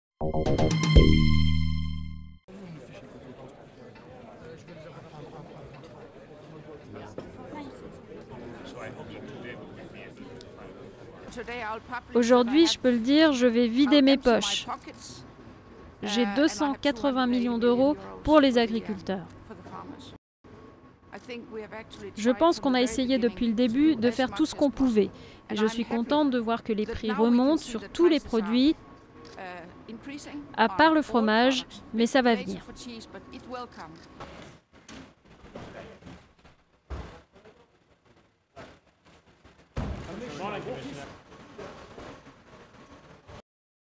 Crise du lait : Bruxelles va débloquer 280 millions d'euros
La commissaire européenne à l'Agriculture Mariann Fischer Boel a annoncé vouloir débloquer 280 millions d'euros en faveur du secteur laitier en crise, peu avant une réunion ministérielle de l'UE.